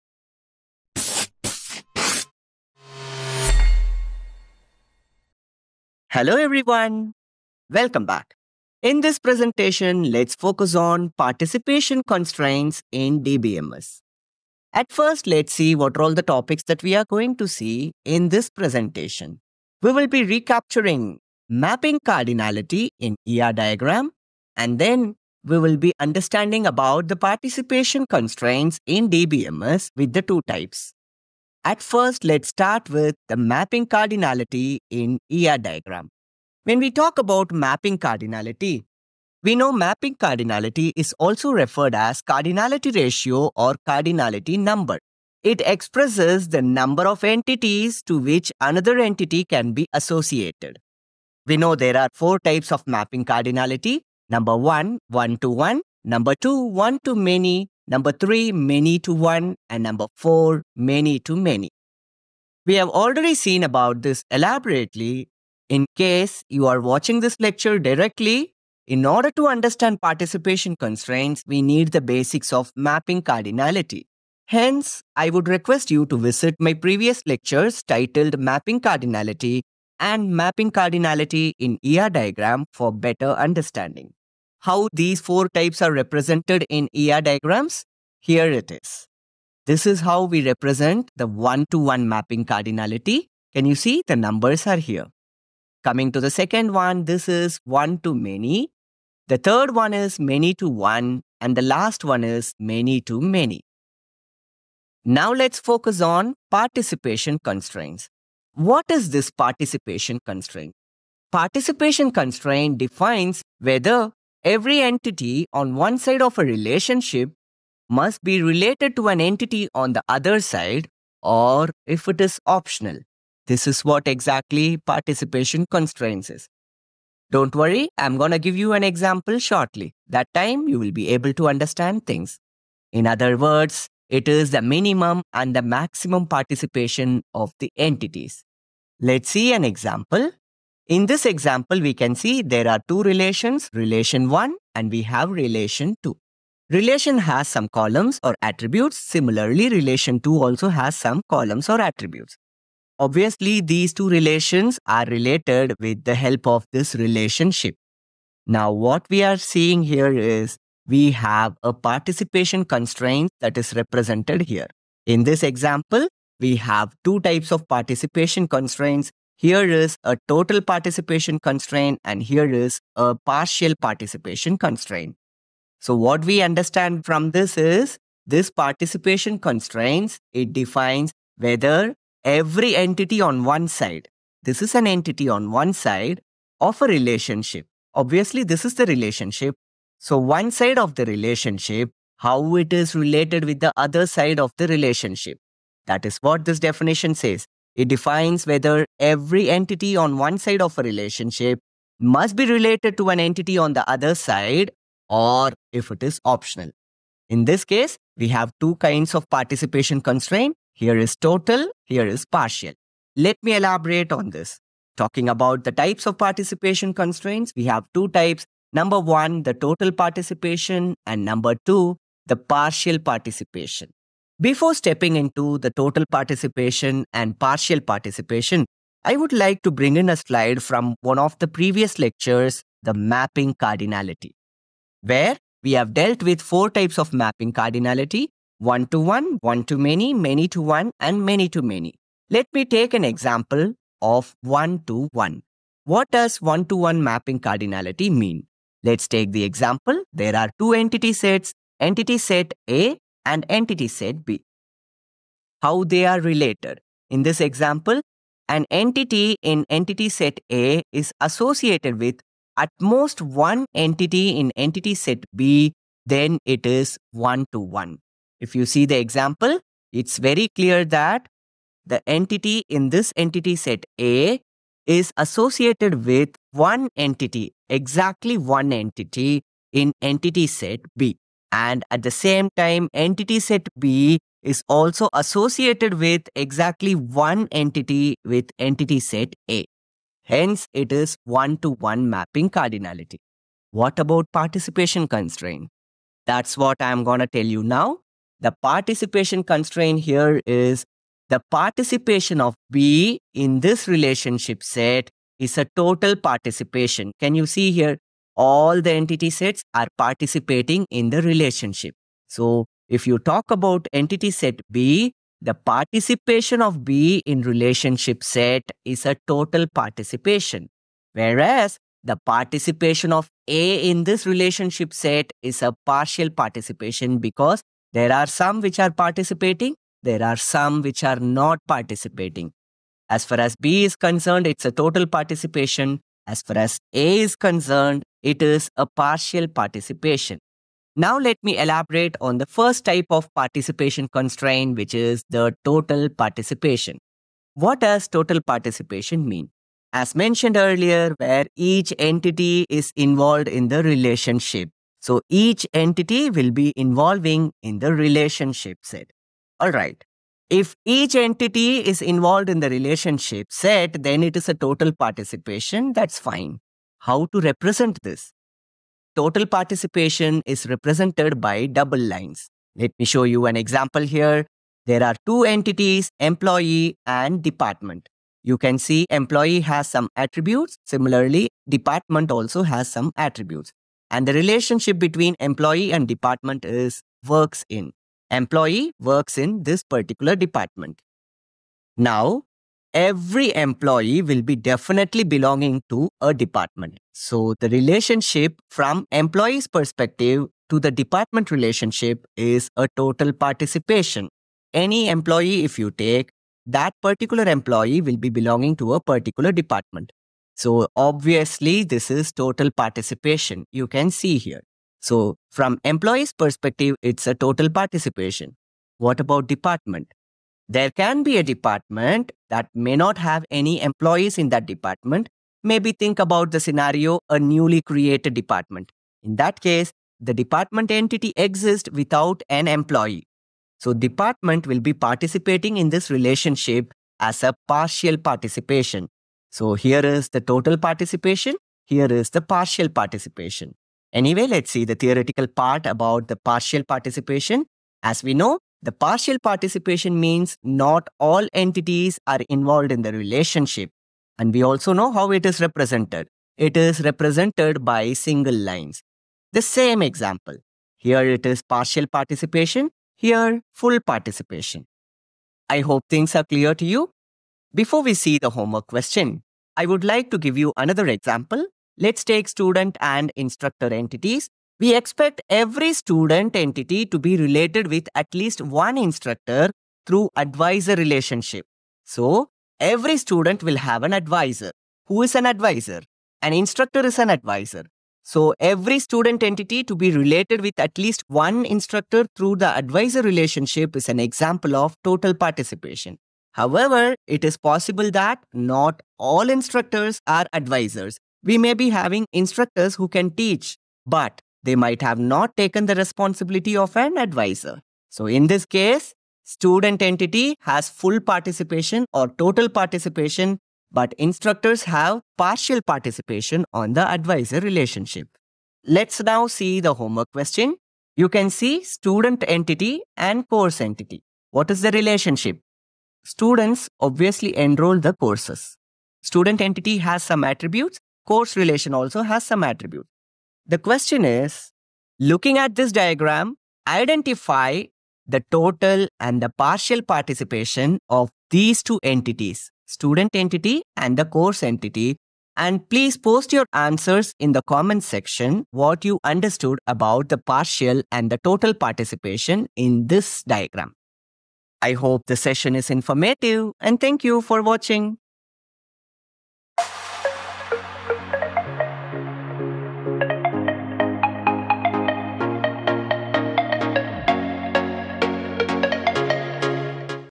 Participation Constraints in DBMS
DBMS: Participation Constraints in DBMS
Topics discussed:
1. Revision of Mapping Cardinality in ER Diagrams.
2. Participation Constraints in DBMS.
3. Total Participation in ER Diagrams.
4. Partial Participation in ER Diagrams.